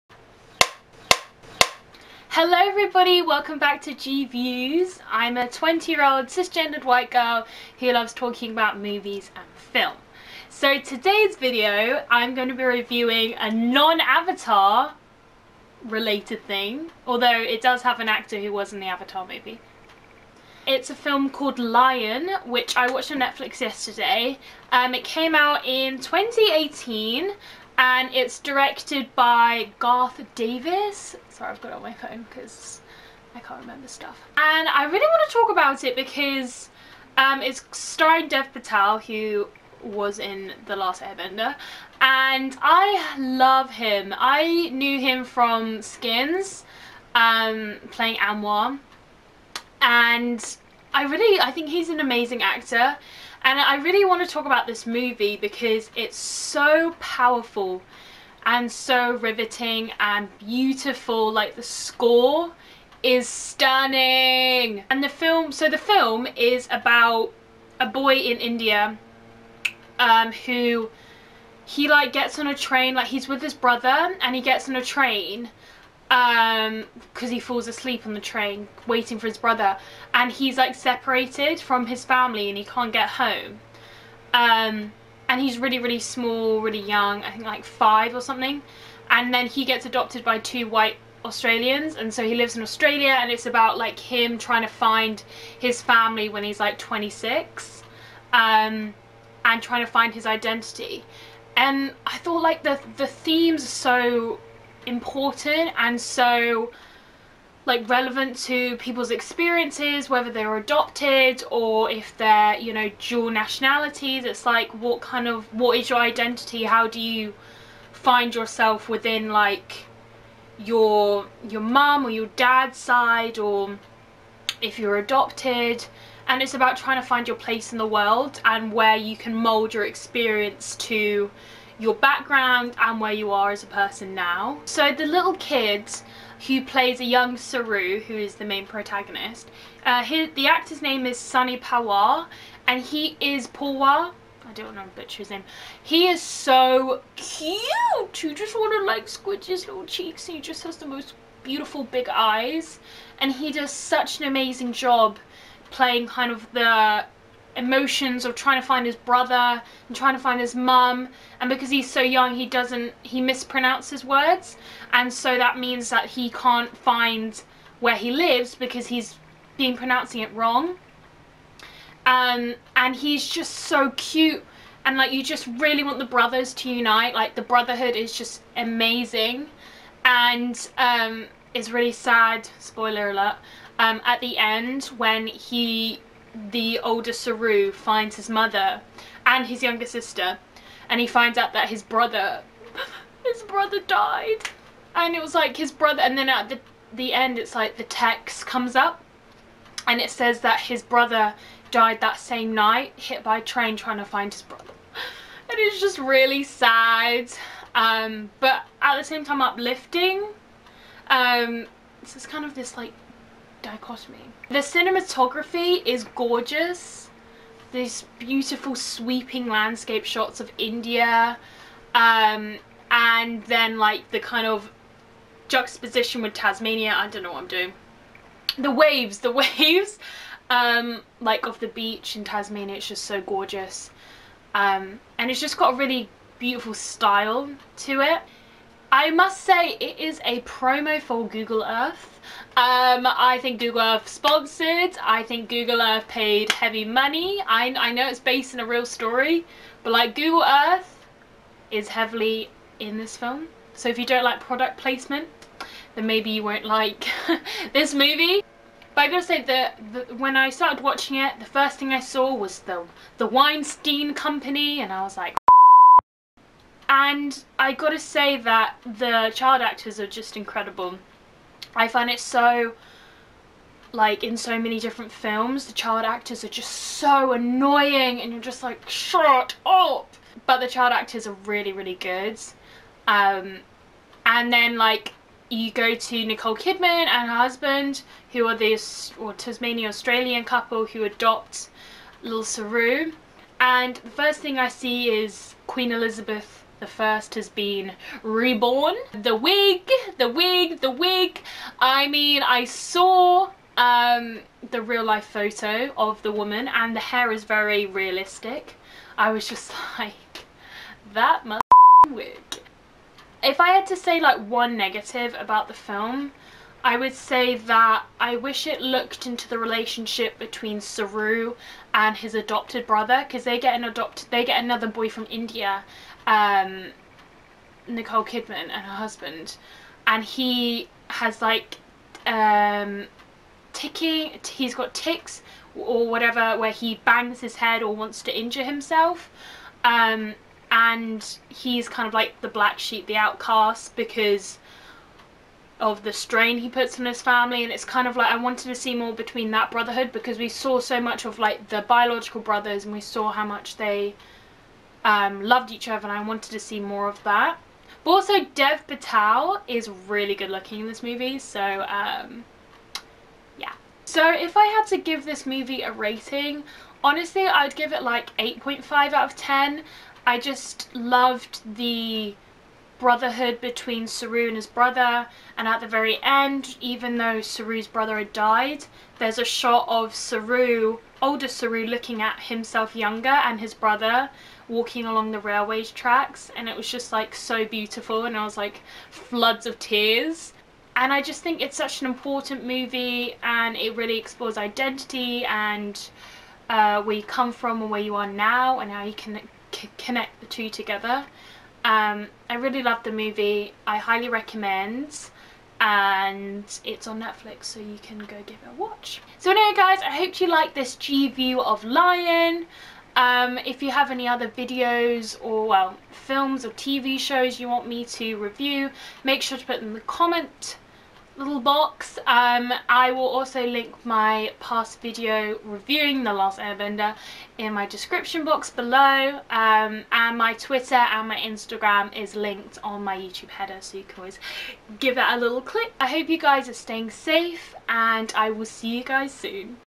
Reviewing: Lion
For today's Ge-views, i'm exploring 2016's Lion: a story of a boy named Saroo and his 20+ year journey to find his birth family. Hope you enjoy!!!

Twitter and Instagram is linked in my Youtube header!!